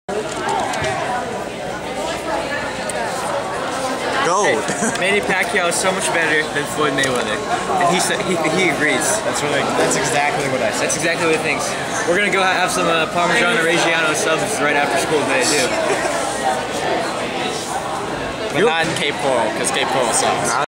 Alex Still Hates Cape Coral... and Mayweather Too
Alex admits that Manny Pacquiao is better than Floyd Mayweather while Ian is good looking.